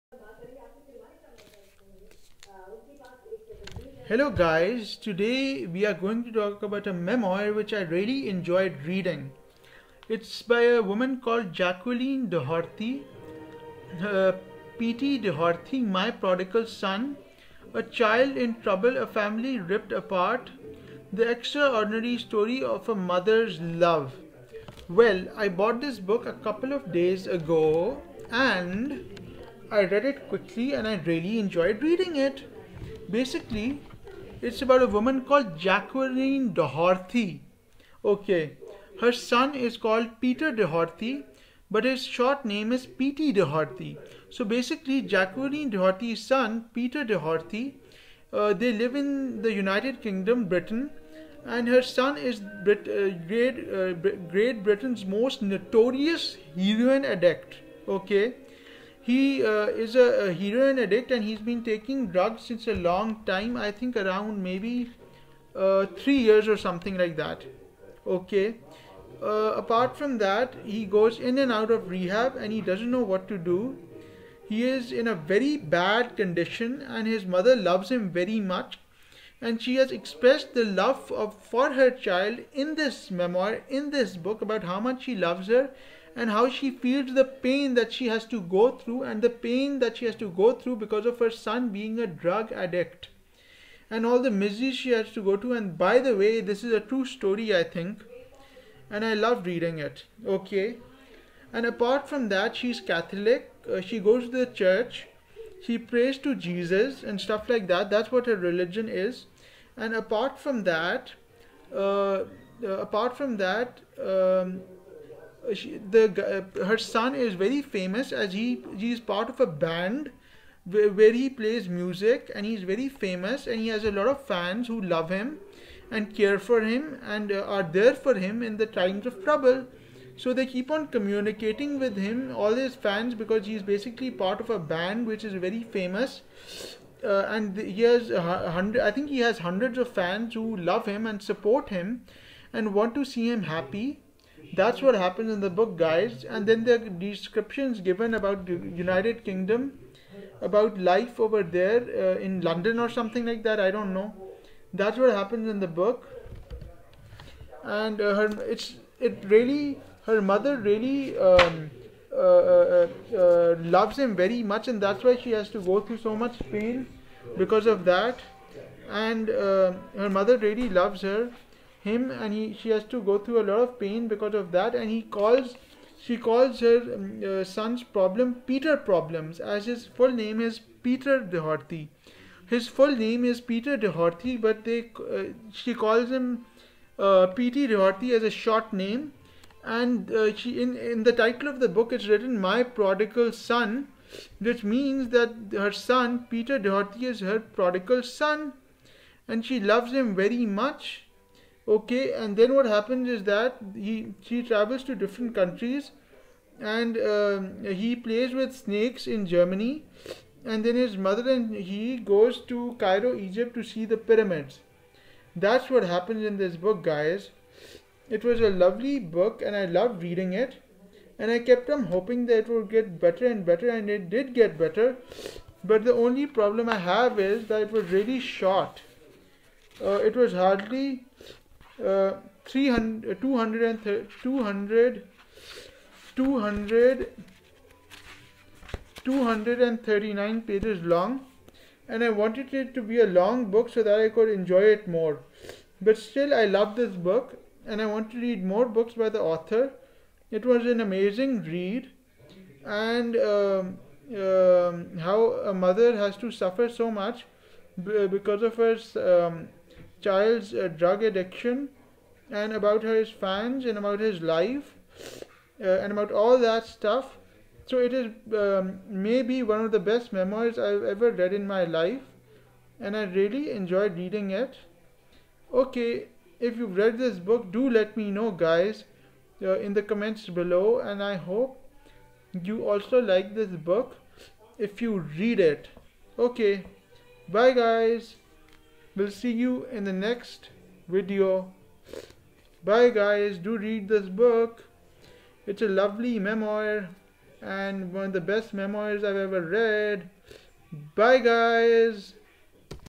Jacqueline Doherty , pate Doherty my prodigal son , a book review!.
A great memoir about England's most notorious drug addict and about how her mother loves him very much and about his life!.